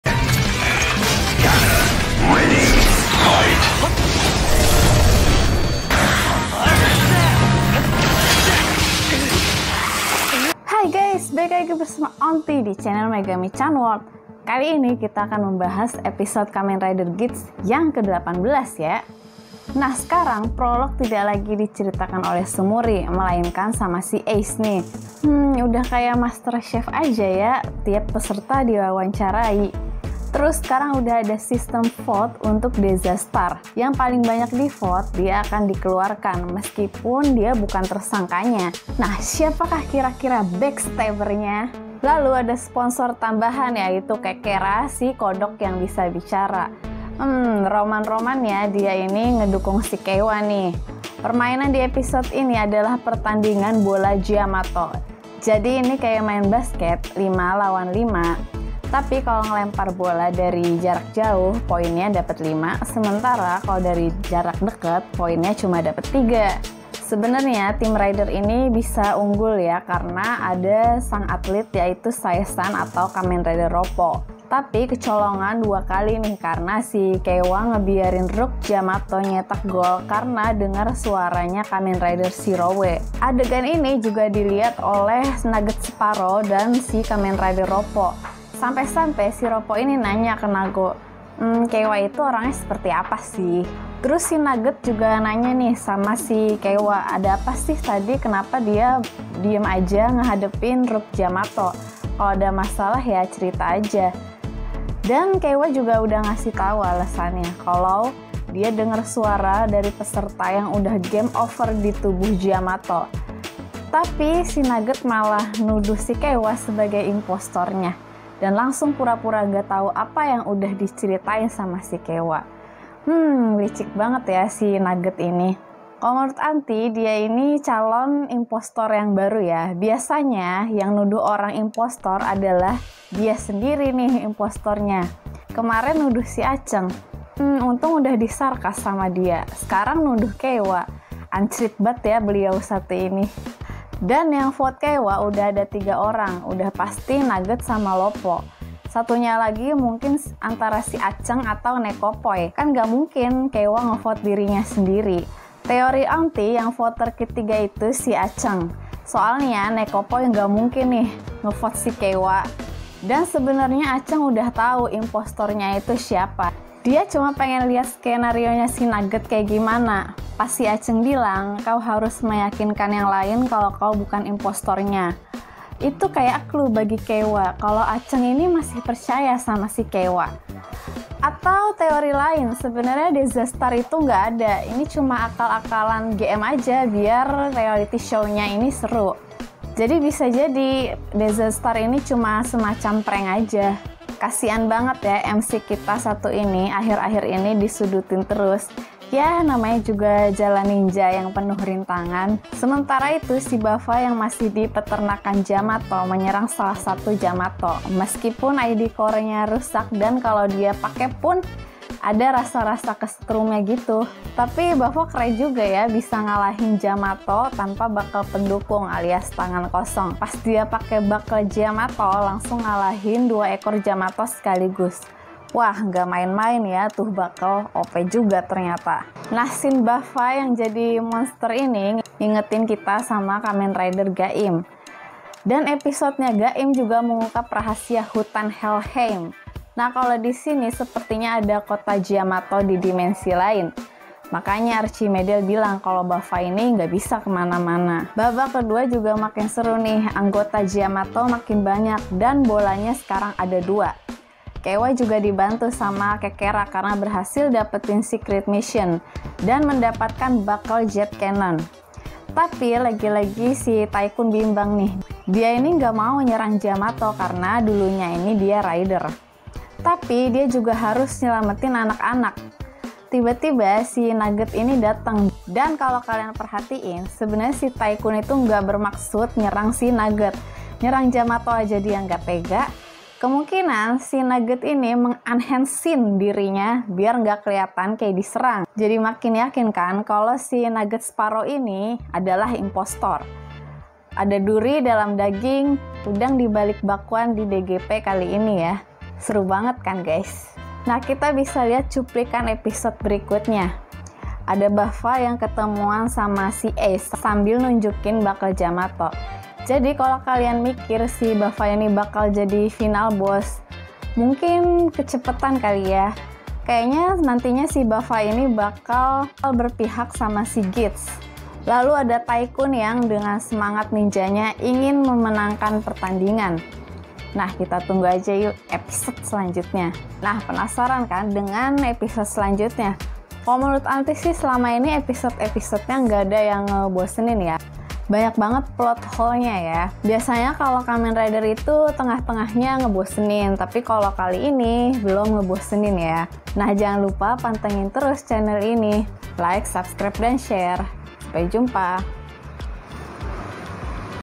KEIWA IMPOSTOR? BUFFA JAMATO FORM OP BANGET! Geats eps 18!
Kali ini kita akan membahas episode kamen rider geats yg ke 18

Untuk keperluan job hubungi kontak diatas ya~